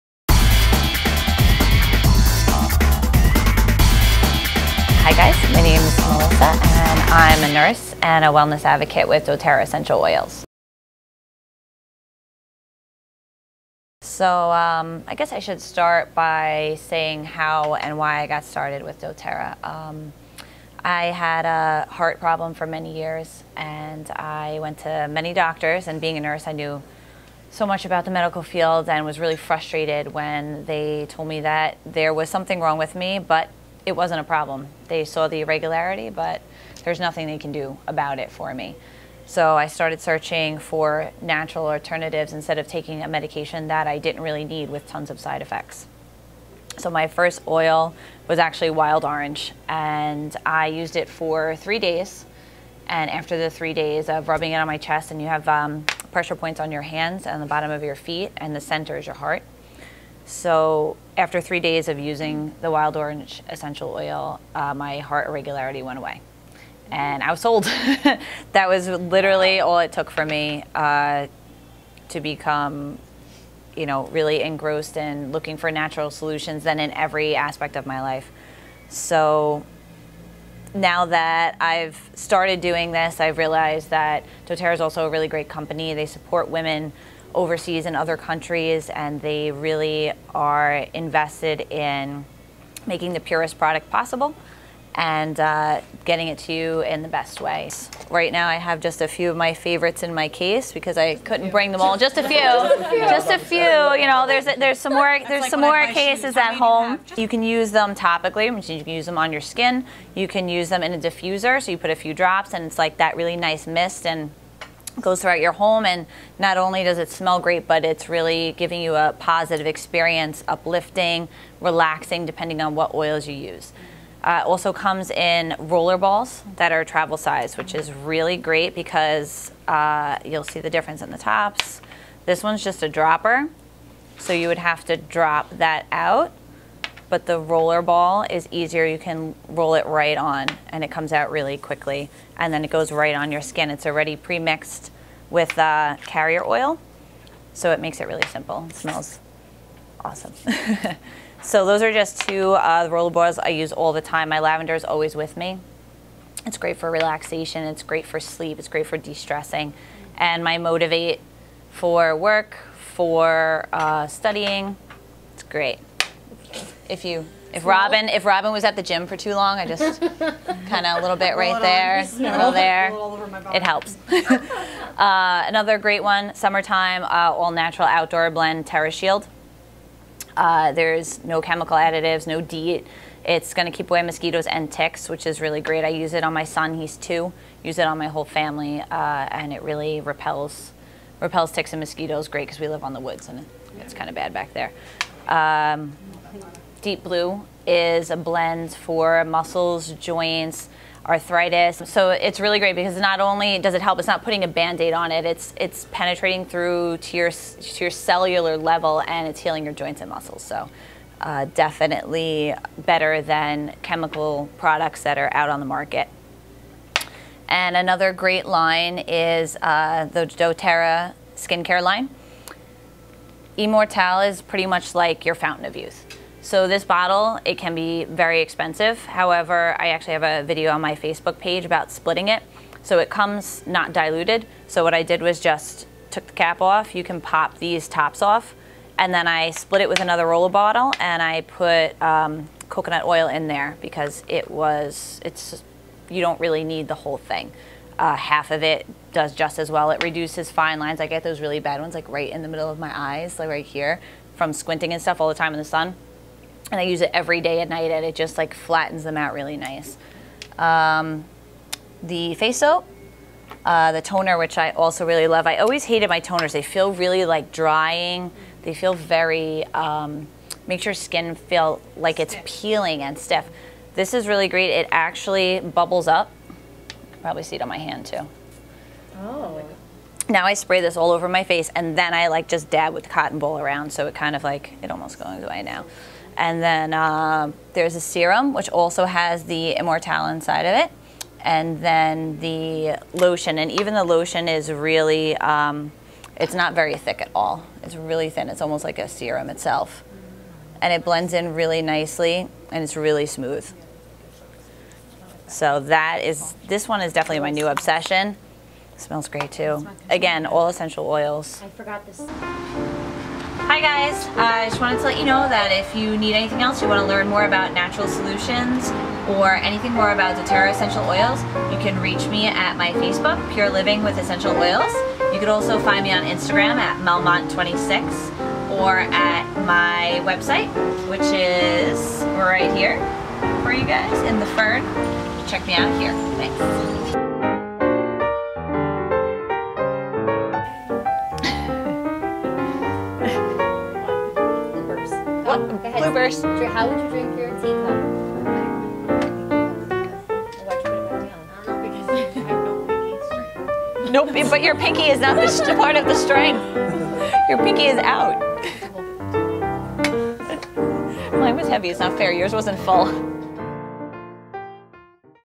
doTERRA review
reviewing some of the doTERRA products that Melissa loves and finds most rewarding in her lifestyle right now.